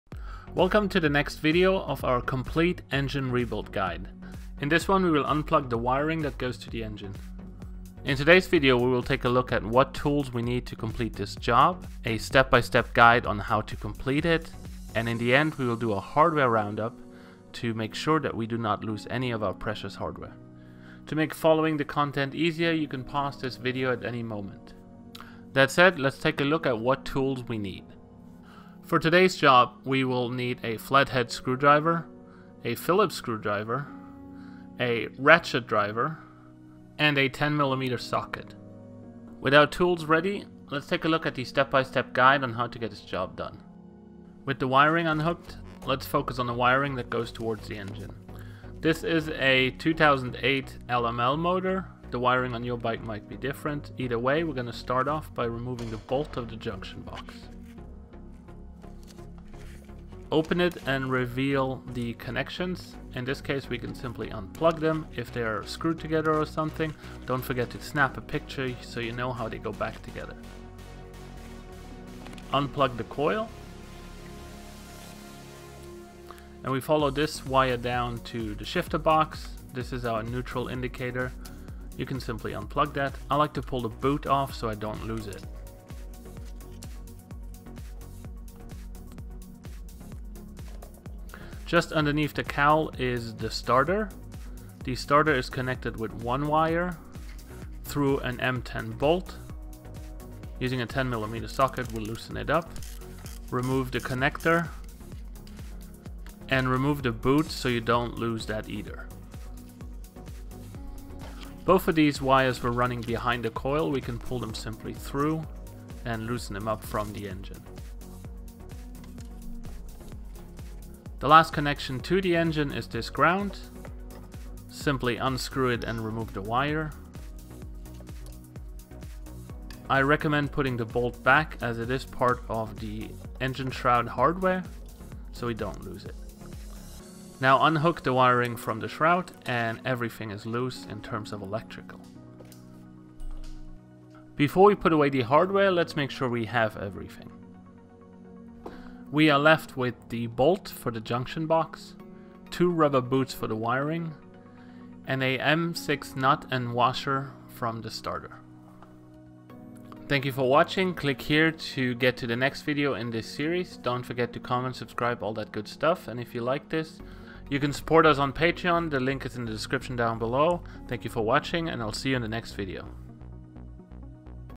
Unplug the engine wiring - Vespa LML Engine rebuild tutorial Part 4
Tools required: 0:24
Step by step guide: 0:39
Hardware roundup: 2:23

Another video in our long series of a full DIY engine rebuild videos, showing you step by step how, when and where. We will turn this LML 150cc 5 Port into a fire breathing 187cc  touring machine.

Part 4: Unplugging the wiring on a LML 2008 engine. Different Vespa engines will vary. But evidently it is the same idea.

This video was created in collaboration with Dr. Schmidt's e-Learning Innovations.